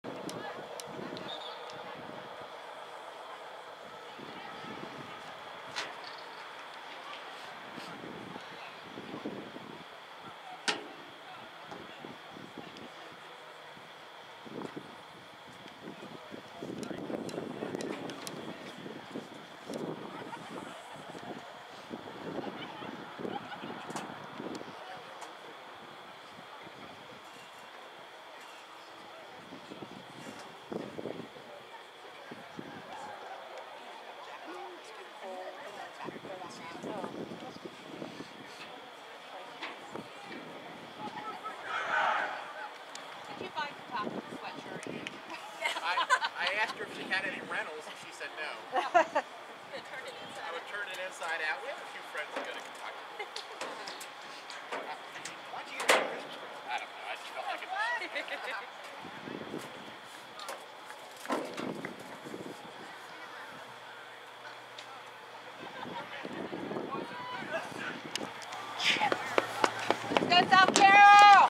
SCHS v Catoctin Boys Varsity Soccer 09-23-2014 Part 07
South Carroll High School @ Catoctin
Boys Varsity Soccer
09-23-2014
SCHS 2, Catoctin 3